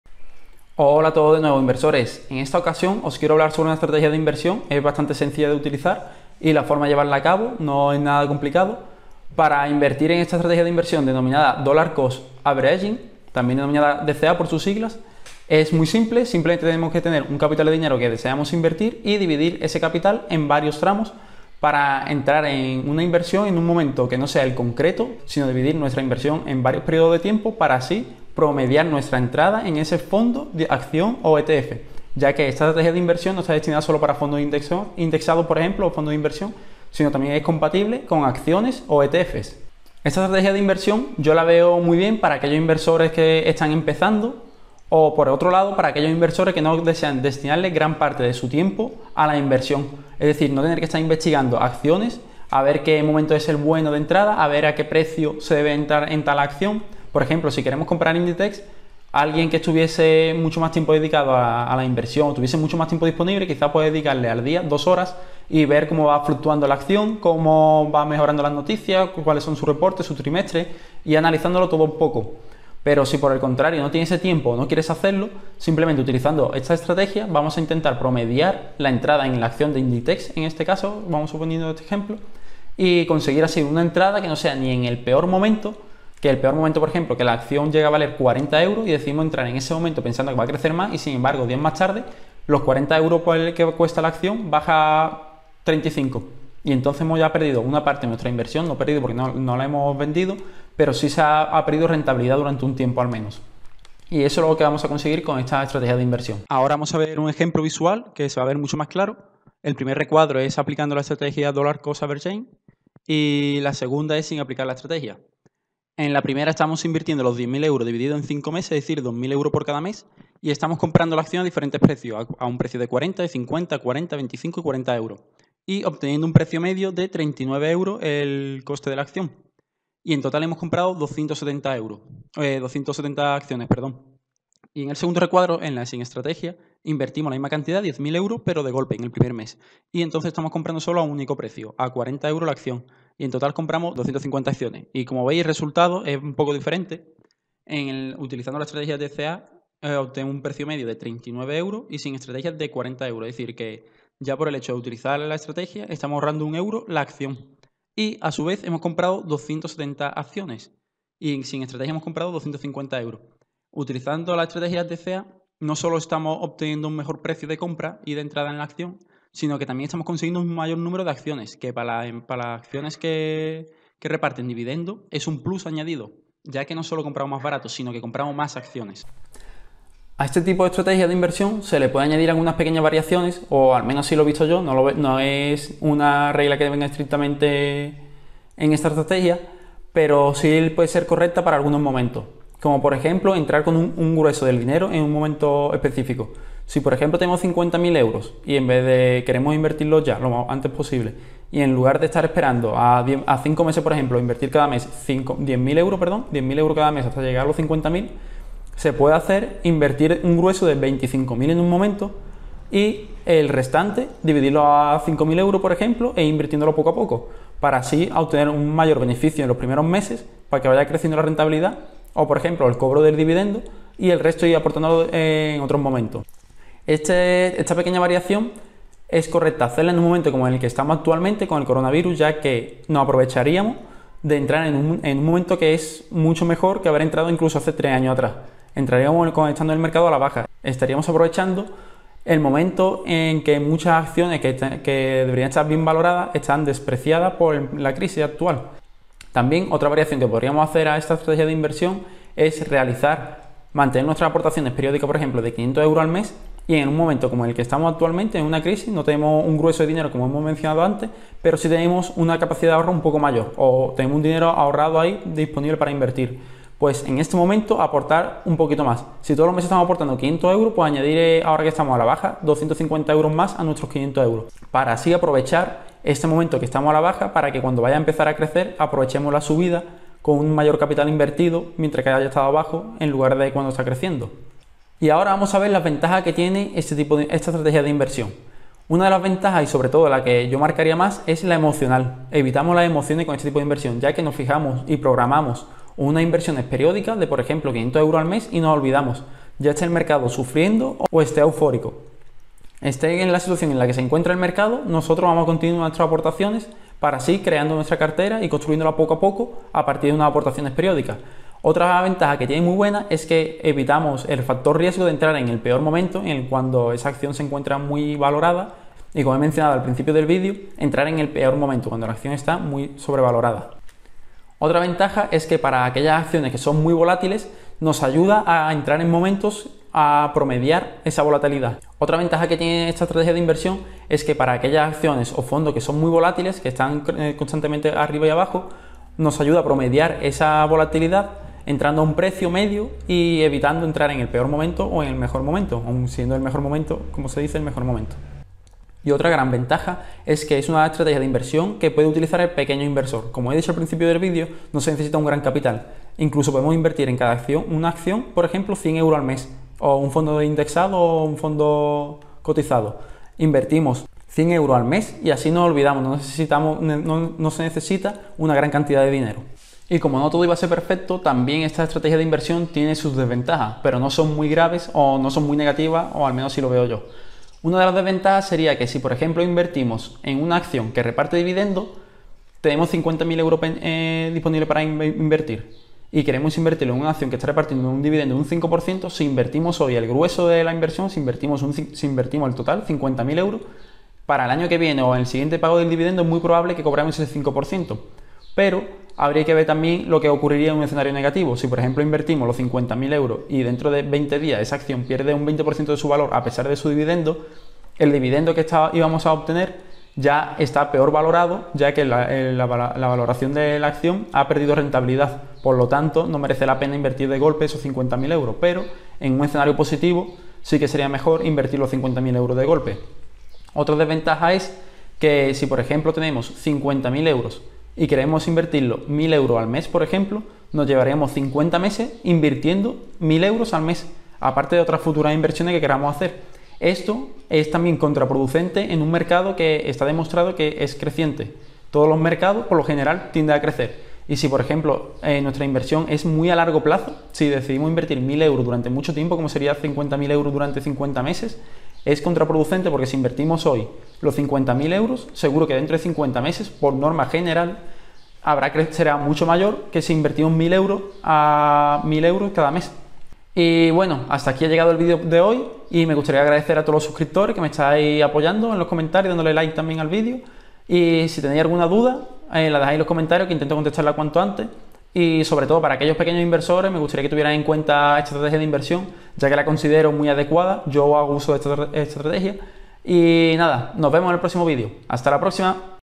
Estrategia de Inversión DCA (Dollar Cost Averaging) ¿Cuándo Invertir?
Como habéis visto esta estrategia es muy sencilla de aplicar y llevar acabo. En última instancia es el propio inversor si decidirá llevarla a acabo. Yo personalmente, en cierto modo, llevo esta estrategia a cabo pero a largo plazo, ya que en fondos indexados hago aportaciones de forma mensual sin fecha de espiración, al menos por ahora.

Lo bueno que tiene esta estrategia es que es compatible con inversiones en acciones, fondos indexados, fondos de gestión activa y fondos cotizados o ETF. Y lo mejor que puede ser usada por cualquier inversor, ya sea pequeño o grande.